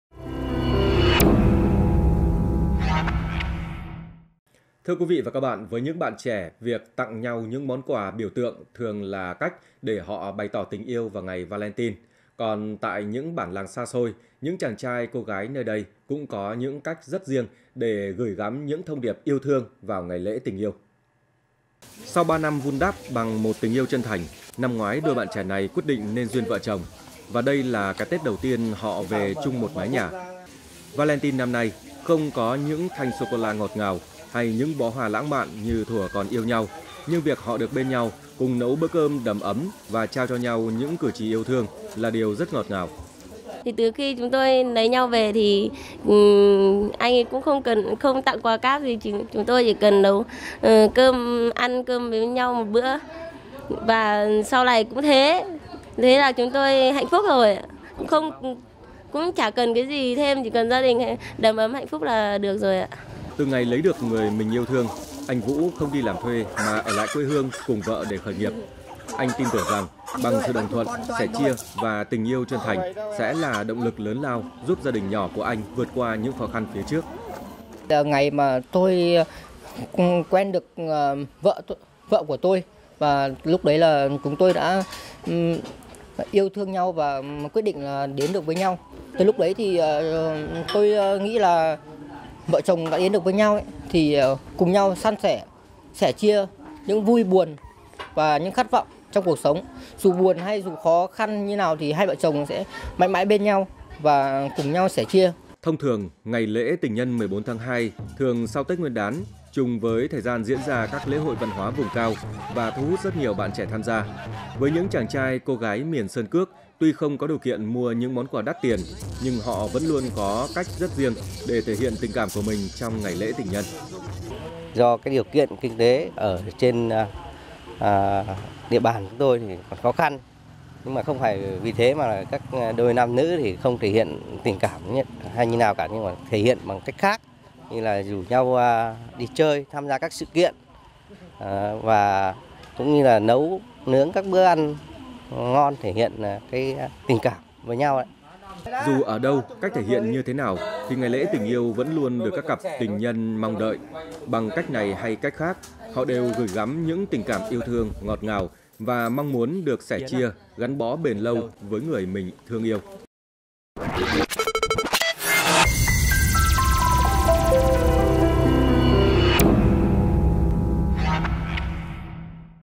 Valentine ở bản vùng cao | THLC
Laocaitv.vn - Với những bạn trẻ việc tặng nhau những món quà biểu tượng thường là cách để họ bày tỏ tình yêu vào ngày Valentine. Còn tại những bản làng xa xôi, những chàng trai, cô gái nơi đây cũng có những cách rất riêng để gửi gắm những thông điệp yêu thương vào ngày lễ tình yêu này.
Đài Phát thanh - Truyền hình Lào Cai